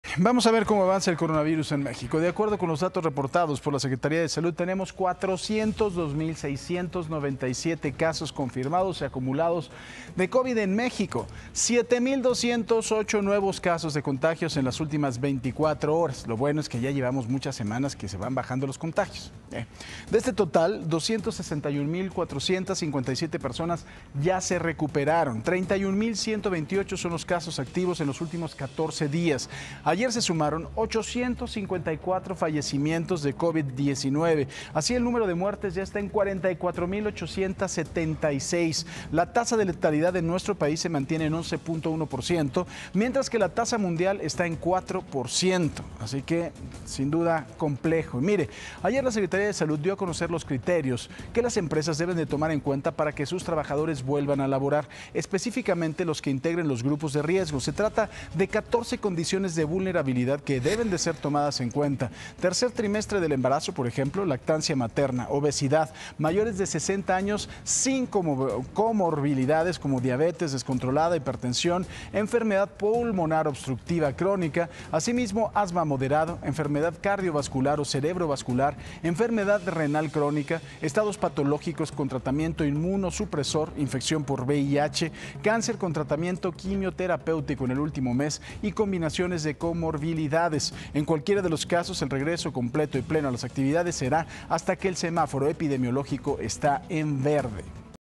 Covid-19 continúa avanzando en México | Noticias con Francisco Zea
De acuerdo con datos reportados por la Secretaría de Salud, en México suman 402 mil 697 casos confirmados de COVID19 y se reportan 44 mil 876 víctimas mortales.